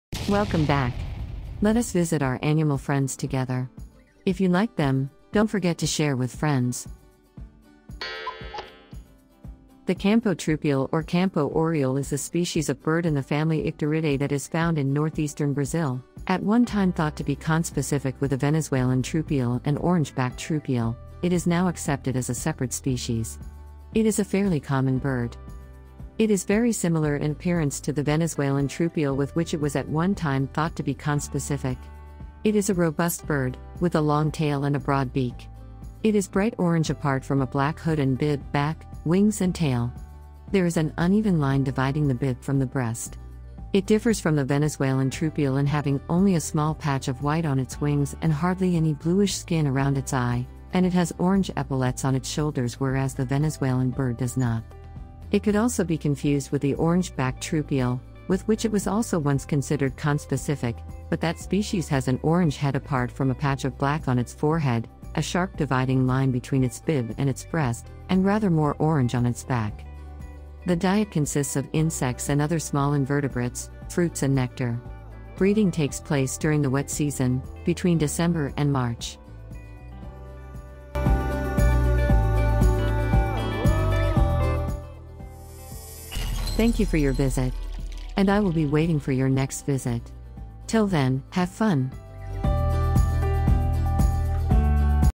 Campo Troupial facts 🦜 Campo Oriole 🦜 found in northeastern Brazil 🇧🇷
birddocumentary campo troupial documentary facts
The campo troupial or campo oriole is a species of bird in the family Icteridae that is found in northeastern Brazil. At one time thought to be conspecific with the Venezuelan troupial and orange-backed troupial, it is now accepted as a separate species. It is a fairly common bird.
It is very similar in appearance to the Venezuelan troupial with which it was at one time thought to be conspecific. It is a robust bird, with a long tail and a broad beak. It is bright orange apart from a black hood and bib, back, wings and tail. There is an uneven line dividing the bib from the breast. It differs from the Venezuelan troupial in having only a small patch of white on its wings and hardly any bluish skin around its eye, and it has orange epaulettes on its shoulders whereas the Venezuelan bird does not. It could also be confused with the orange-backed troupial, with which it was also once considered conspecific, but that species has an orange head apart from a patch of black on its forehead, a sharp dividing line between its bib and its breast, and rather more orange on its back.
The diet consists of insects and other small invertebrates, fruits and nectar. Breeding takes place during the wet season, between December and March.